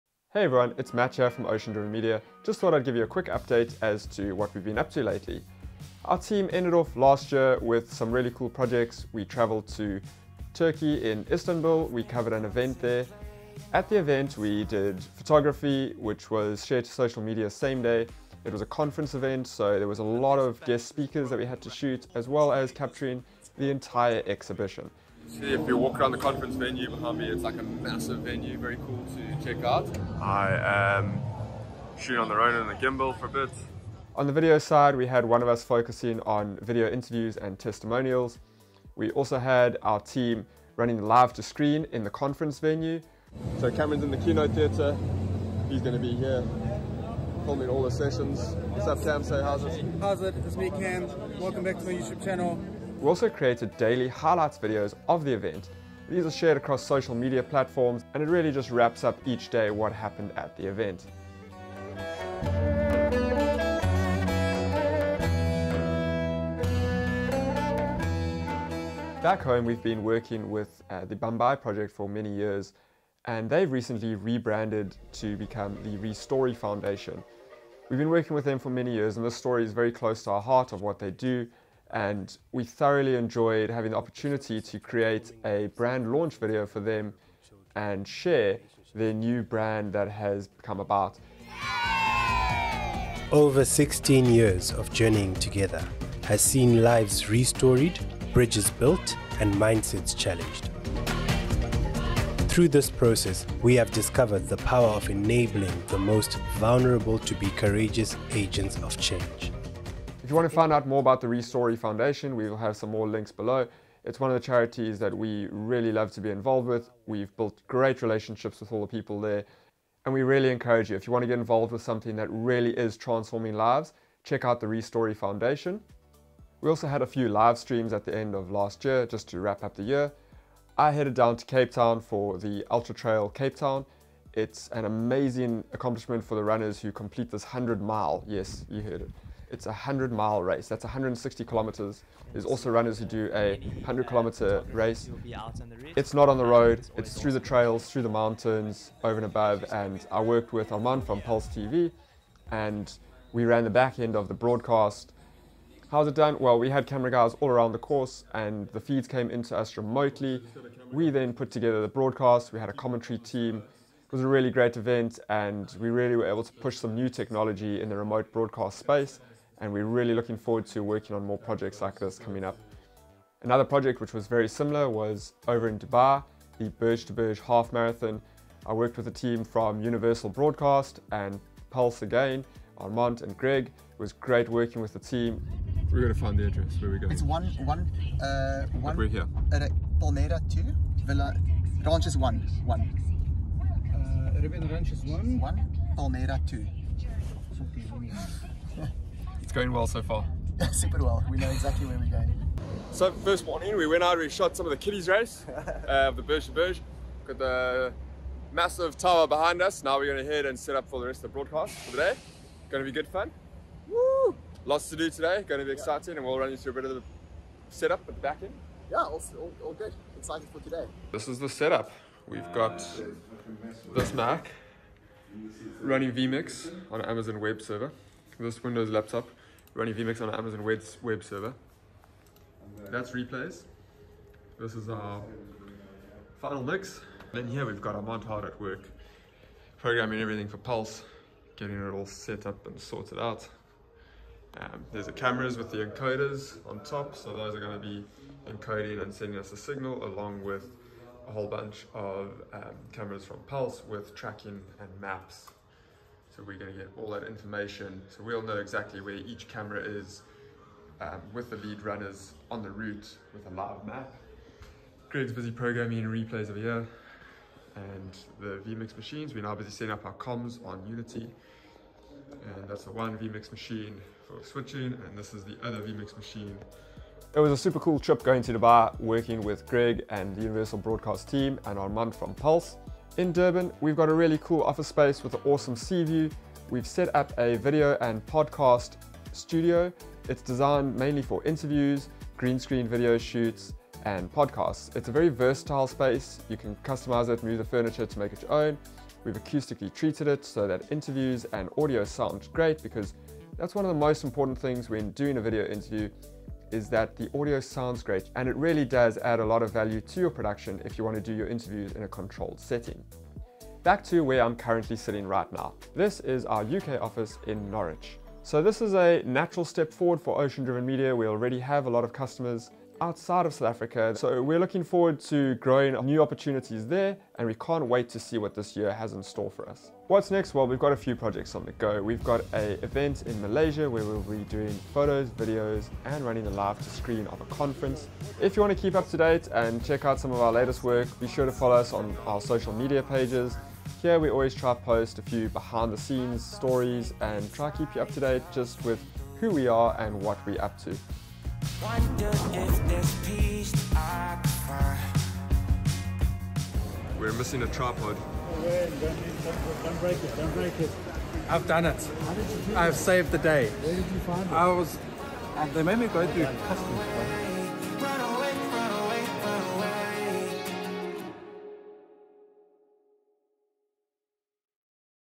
What We Do | Photography, Videography and Live Streaming Update
Dive into our recent project highlights! As a passionate photography, videography, and Live Streaming production team, we’ve captured moments that resonate. From behind-the-scenes magic to stunning visual storytelling, we’re thrilled to share our work with you. 📸🎥

Go see the amazing work The ReStory Foundation has been up